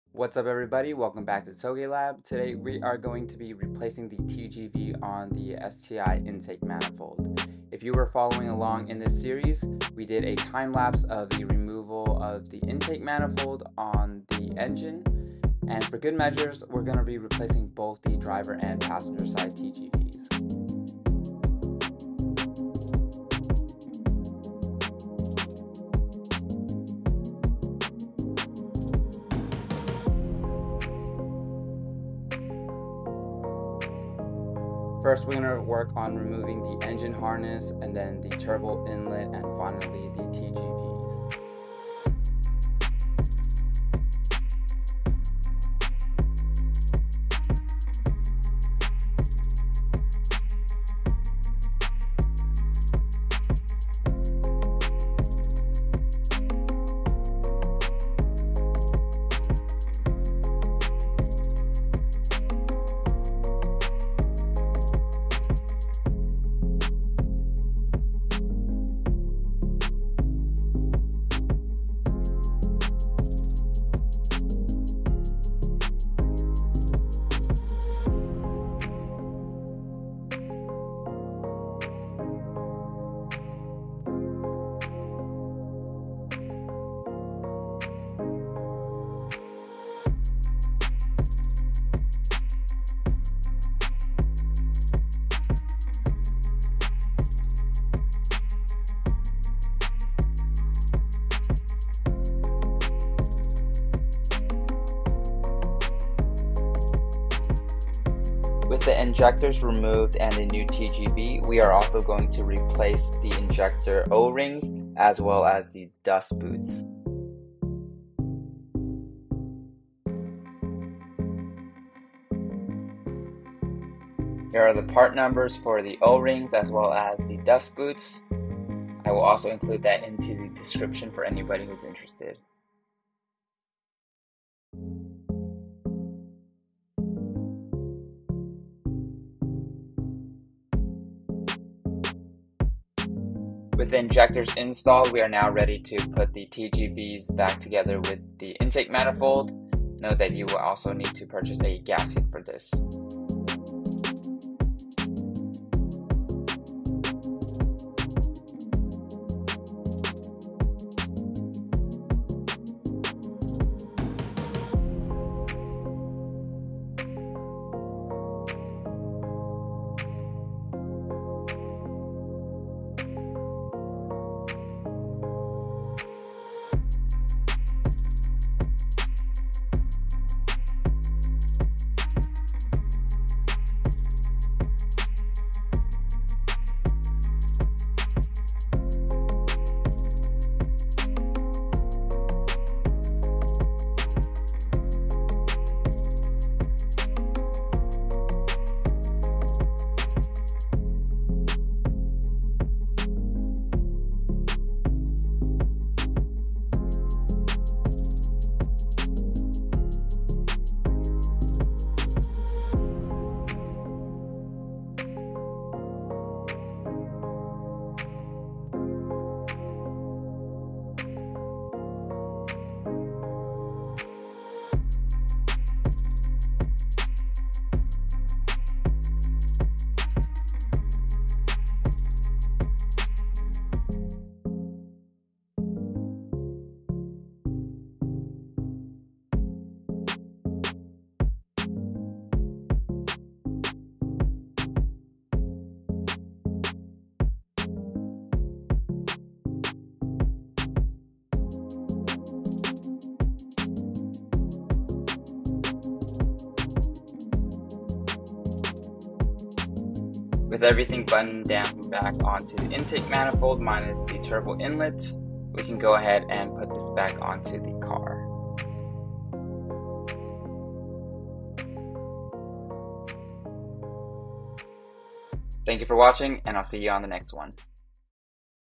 STI TGV Replacement Part 3: Replacing TGV
2013 STI passenger side TGV failed. So we decided to replace both sides as a precaution. The last thing we want is to remove everything again to replace the other side.

This is Part 3 of  the series and we are finally at the point where we can replace the TGVs!

With the intake manifold removed we can replace the TGV with new ones as well as replacing the intake manifold to TGV gasket and TGV to the heads.

We also replaced the injector seals and dust boots for good measure.

OEM parts used:
16175AA243 Gasket
14011AB931 Intake Manifold
14011AB940 Intake Manifold
16395aa020 Fuel Injector Seal
16608ka000 Fuel Injector Seal
16698aa110 Fuel Injector O-Ring

Stay tune for part 4